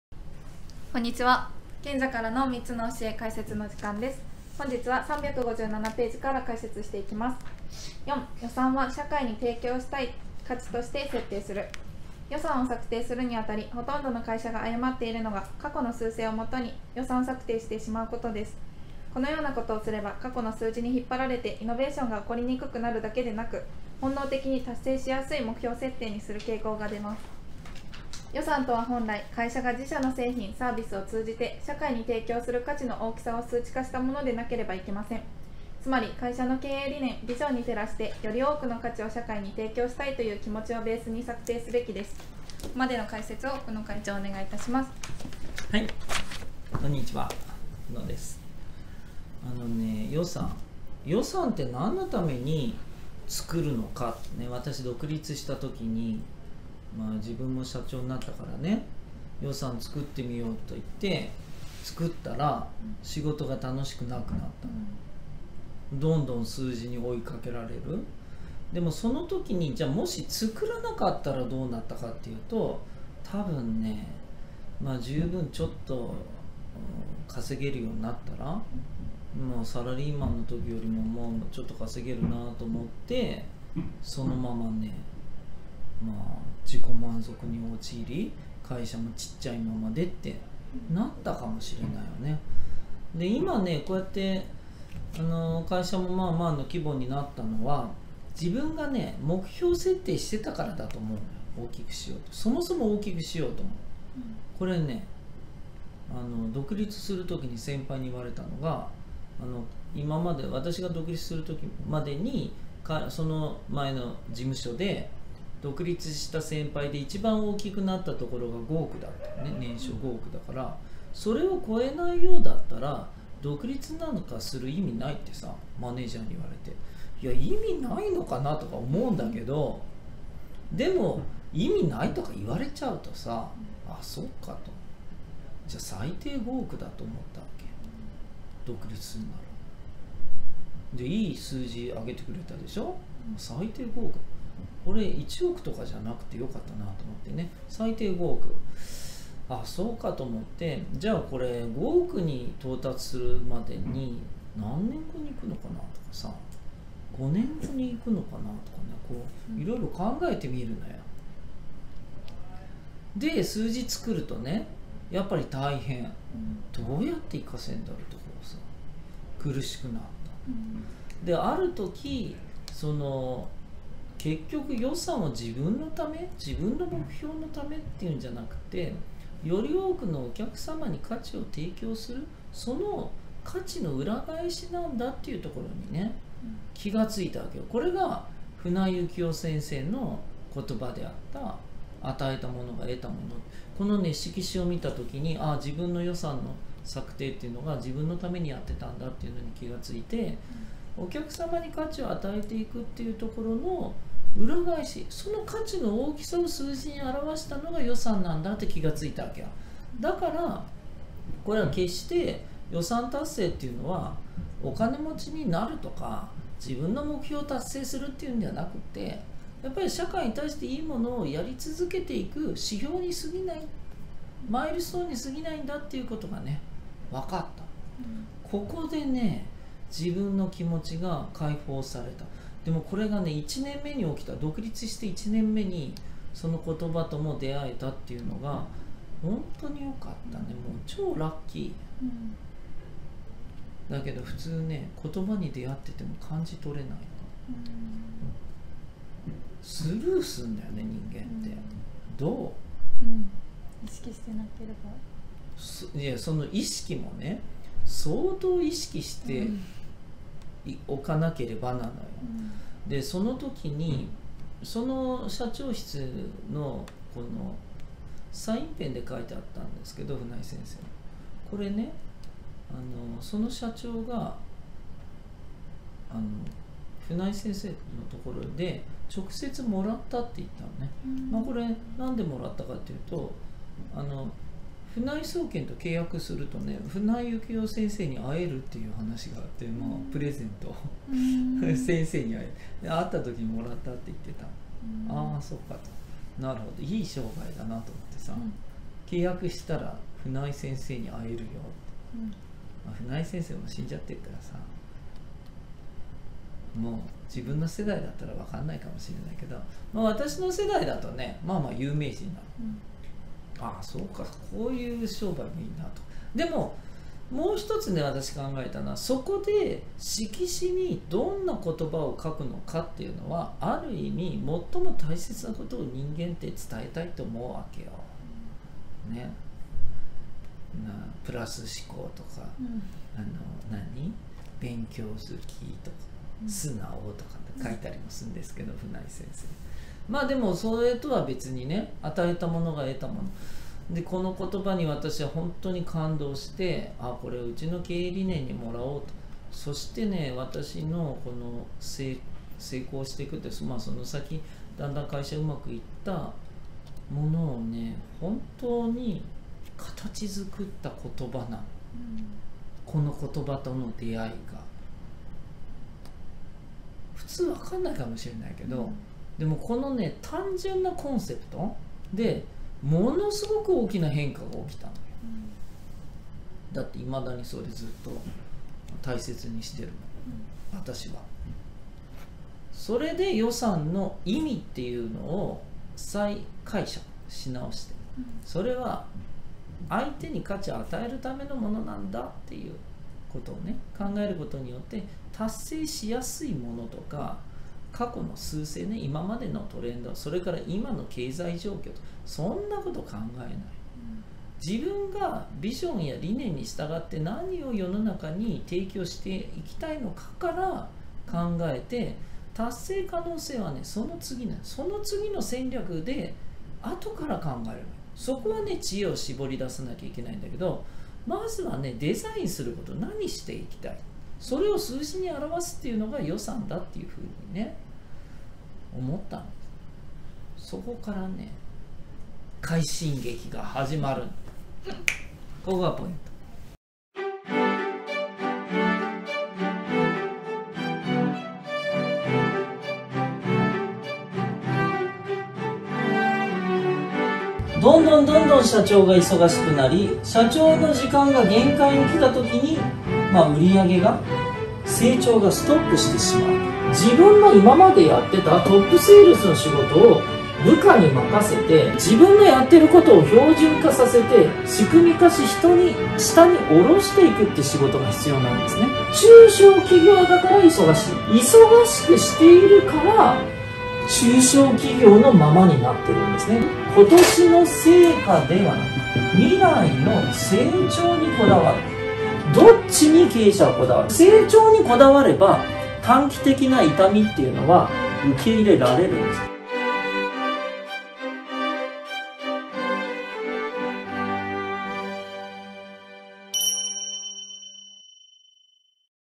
【第267話】予算は社会に提供したい価値として設定する Part1 ｜『賢者からの三つの教え』著者解説‼
予算の組み方を間違えると会社にとって大きな命取りとなります。
正しく予算を設定するために、何を基準に組み立てていくべきかについてお話していきます。

話題のビジネス書『賢者からの三つの教え』を著者自ら徹底解説！
今回の解説箇所：p357～358
0:00 挨拶
0:09 朗読
0:45  解説
「第8章　達成」

■海外進出・海外実務セミナーはこちら

■海外ビジネス最新情報をお届け！無料メルマガはこちら